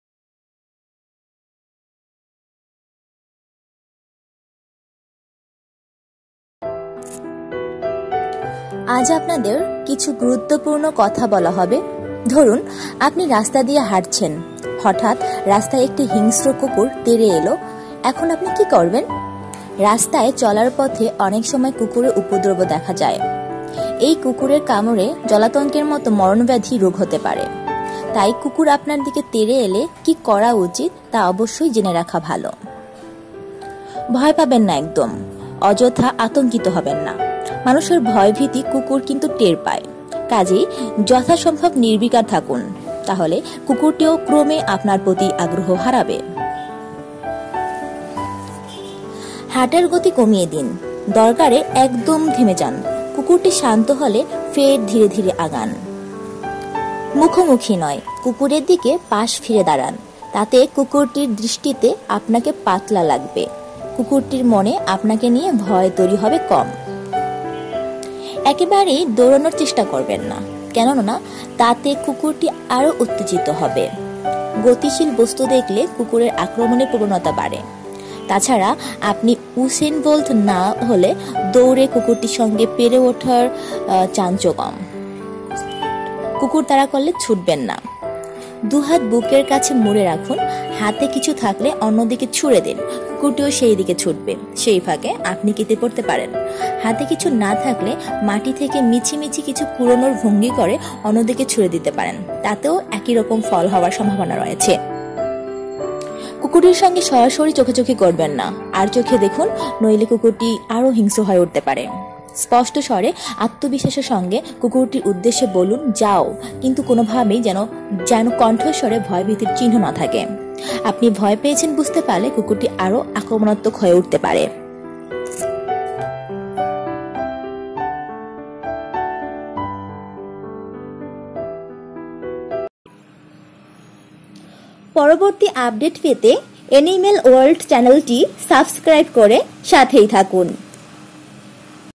রাস্তায় হিংস্র কুকুর তারা করলে কি করবেন? What do they do if they are violent dogs on the street?
ধরুন আপনি রাস্তা দিয়ে হাটছেন হঠাৎ রাস্তায় একটি হিংস্র কুকুর তেড়ে এলো এখন আপনি কি করবেন?

রাস্তায় চলার পথে অনেক সময় কুকুরের উপদ্রব দেখা যায়। এই কুকুরের কামড়ে জলাতঙ্কের মতো মরণ ব্যাধি রোগ হতে পারে। তাই কুকুর আপনার দিকে তেড়ে এলে কি করা উচিত, তা অবশ্যই জেনে রাখা ভালো।

* ভয় পাবেন না একদম। অযথা আতঙ্কিত হবেন না। মানুষের ভয়-ভীতি কুকুর কিন্তু টের পায়। কাজেই যথাসম্ভব নির্বিকার থাকুন। তাহলে কুকুরটিও ক্রমে আপনার প্রতি আগ্রহ হারাবে।

* হাঁটার গতি কমিয়ে দিন। দরকারে একদম থেমে যান। কুকুরটি শান্ত হলে ফের ধীরে ধীরে আগান।

* মুখোমুখি নয়, কুকুরের দিকে পাশ ফিরে দাঁড়ান। তাতে কুকুরটির দৃষ্টিতে আপনাকে পাতলা লাগবে। কুকুরটির মনে আপনাকে নিয়ে ভয় তৈরি হবে কম।

* একেবারেই দৌড়ানোর চেষ্টা করবেন না। কেননা তাতে কুকুরটি আরও উত্তেজিত হবে। গতিশীল বস্তু দেখলে কুকুরের আক্রমণের প্রবণতা বাড়ে। তা ছাড়া আপনি উসেইন বোল্ট না হলে দৌড়ে কুকুরটির সঙ্গে পেরে ওঠার চান্সও কম! কুকুর তাড়া করলে ছুটবেন না।

* দু’হাত বুকের কাছে মুড়ে রাখুন।

* হাতে কিছু থাকলে অন্য দিকে ছুড়ে দিন। কুকুরটি সেই দিকে ছুটবে। সেই ফাকে আপনি কেটে পড়তে পারেন। হাতে কিছু না থাকলে মাটি থেকে মিছিমিছি কিছু কুড়োনোর ভঙ্গি করে অন্য দিকে ছুড়ে দিতে পারেন। তাতেও একই রকম ফল হওয়ার সম্ভাবনা রয়েছে।

* কুকুরটির সঙ্গে সরাসরি চোখাচোখি করবেন না। আড়চোখে দেখুন। নইলে কুকুরটি আরও হিংস্র হয়ে উঠতে পারে।

* স্পষ্ট স্বরে আত্মবিশ্বাসের সঙ্গে কুকুরটি উদ্দেশে বলুন 'যাও' কিন্তু কোনোভাবেই যেন যেন কণ্ঠস্বরে ভয়-ভীতির চিহ্ন না থাকে। আপনি ভয় পেয়েছেন বুঝতে পারলে কুকুরটি আরও আক্রমণাত্মক হয়ে উঠতে পারে।